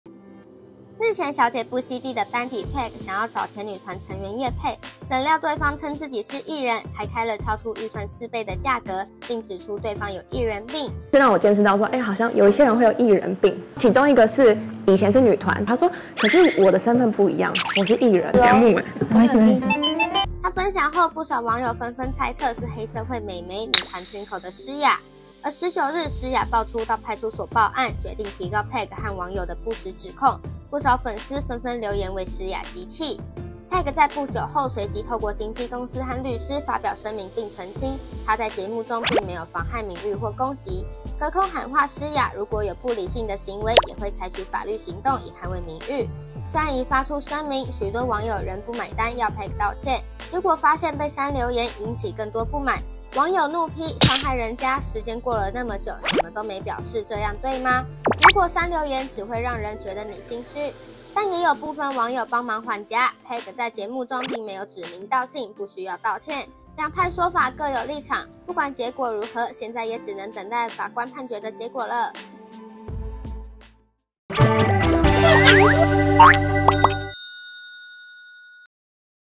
詩亞遭爆「藝人病」報警不忍了《不熙娣》班底遭公審逼道歉
詩亞遭爆「藝人病」報警不忍了
《不熙娣》班底遭公審逼道歉